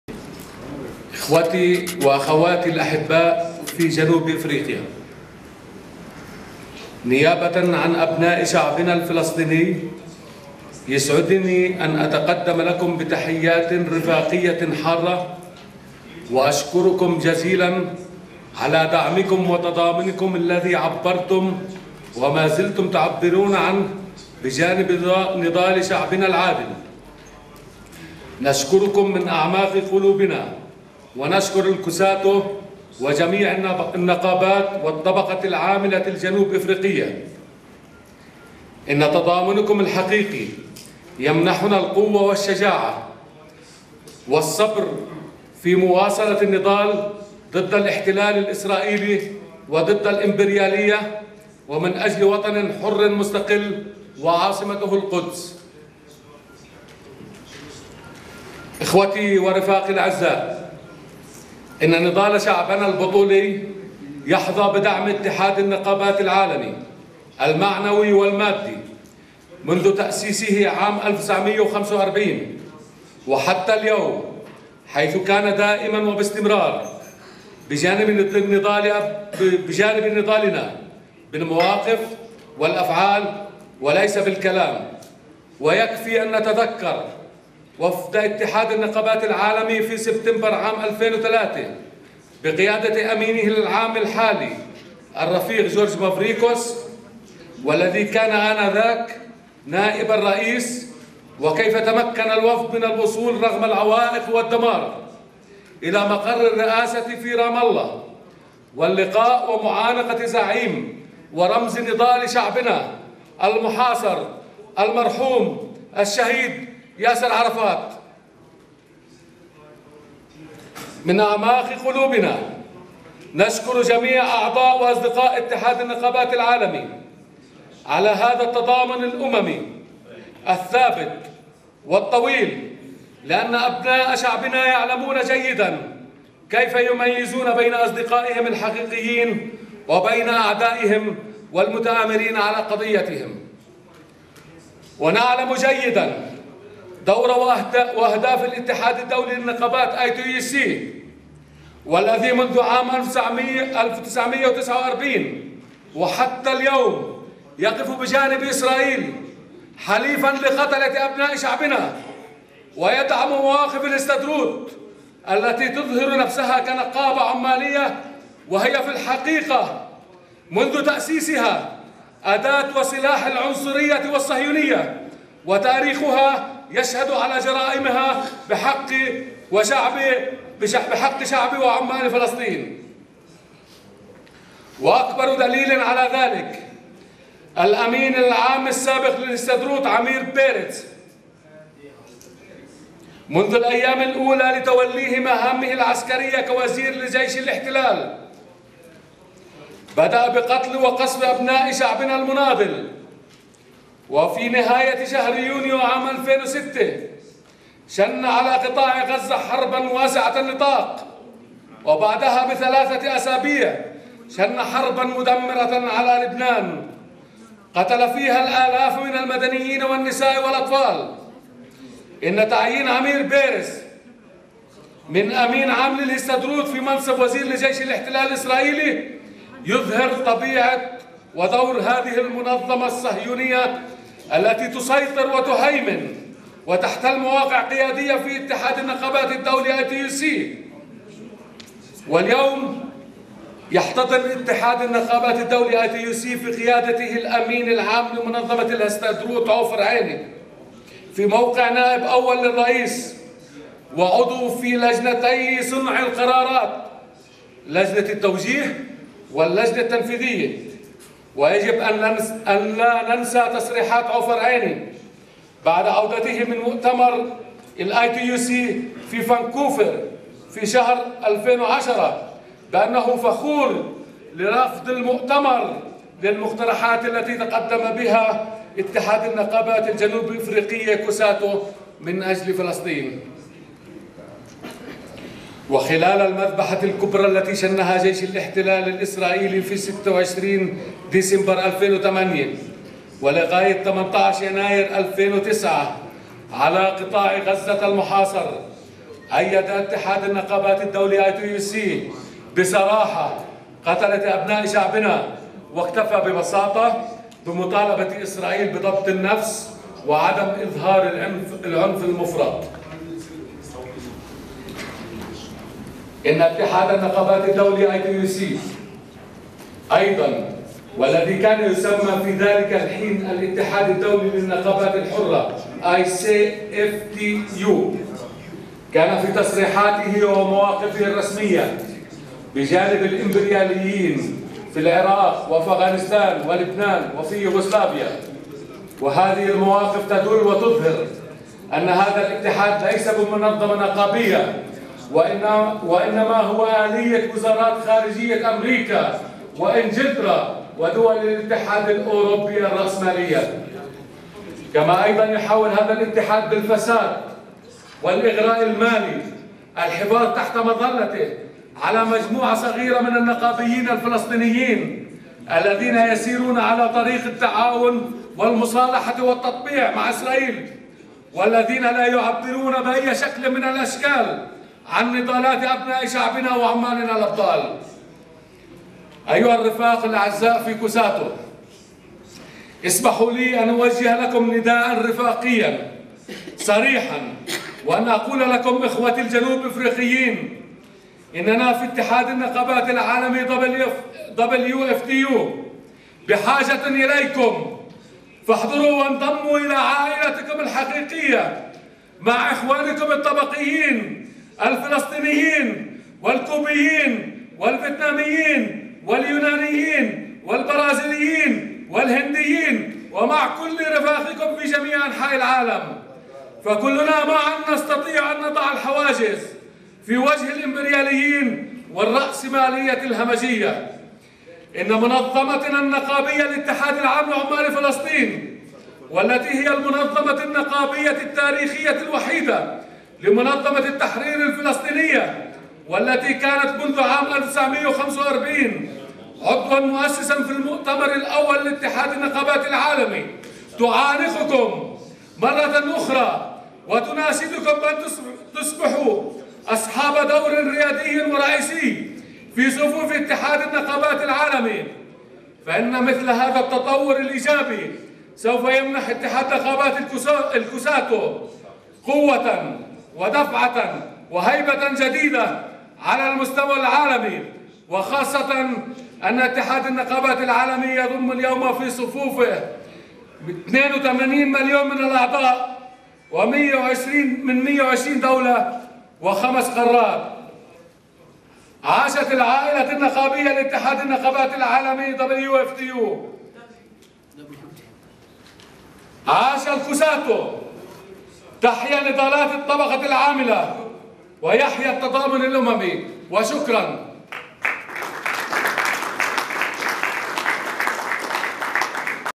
VI Presidential Council Meeting: Speech by Mohammed Iqnaibi, Representative of the GUPW in the WFTU
VI Presidential Council Meeting: Speech by Mohammed Iqnaibi, Representative of the General Union of Palestine Workers in the WFTU